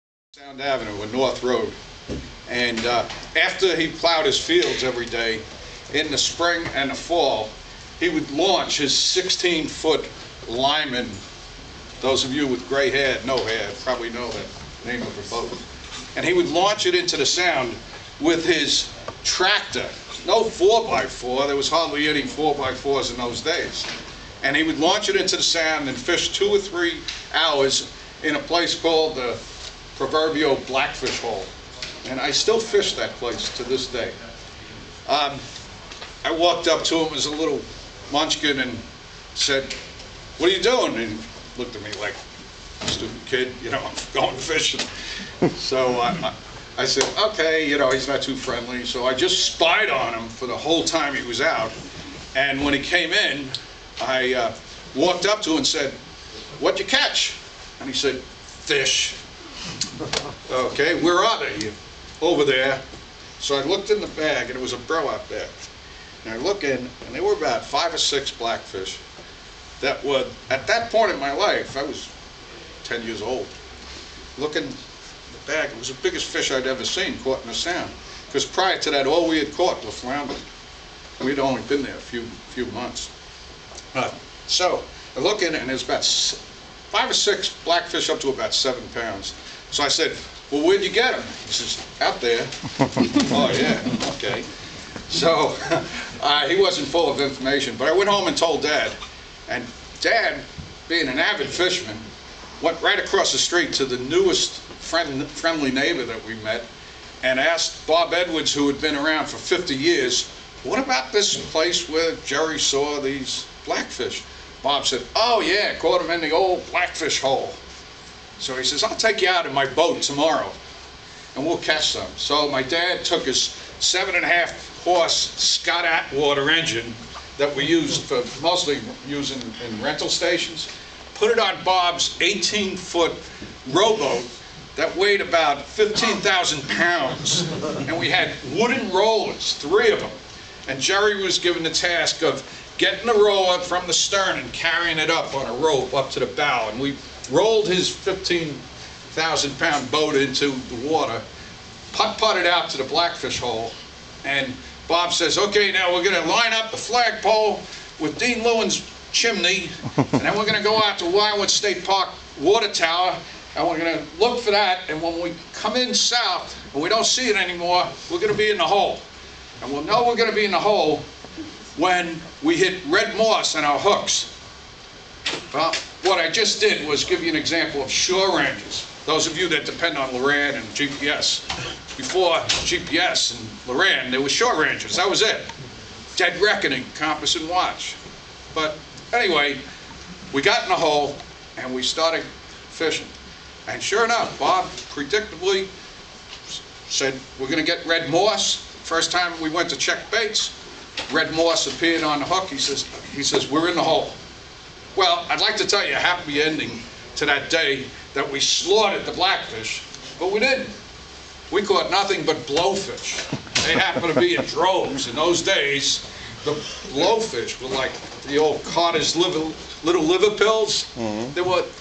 BEFORE THERE WAS GPS
Capt.Jerry Explaining how he use to find Blackfish before GPS